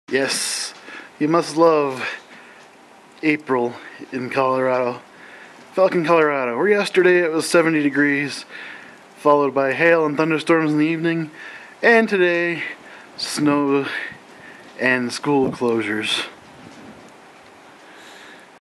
Falcon, Colorado Snow Storm on April 23, 2010
Just thought I'd post a quick little video of the snow storm that is hitting us today. Figured the big flakes of snow would translate well to being recorded on my HD camera.